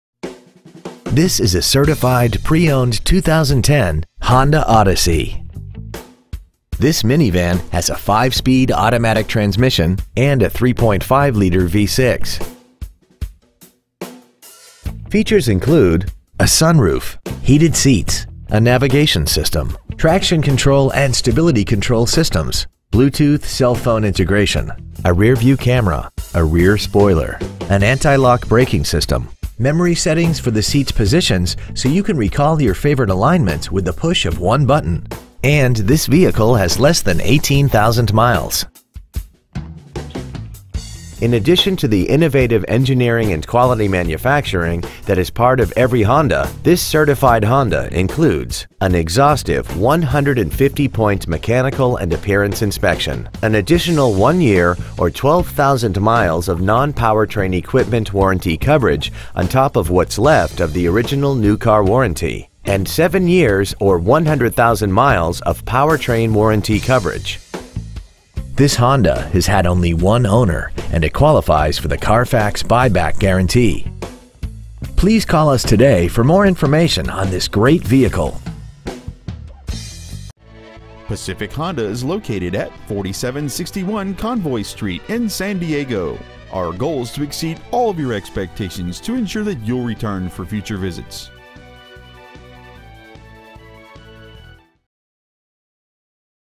2010 Honda Odyssey San Diego CA
Year : 2010
 Make : Honda
 Model : Odyssey
 Engine : 3.5L V6
 Trans . : Automatic 5-Speed
 Exterior : Taffeta White
 Miles : 17,789
 Interior : Ivory w/Leather Seat Trim
 Stock : 122866A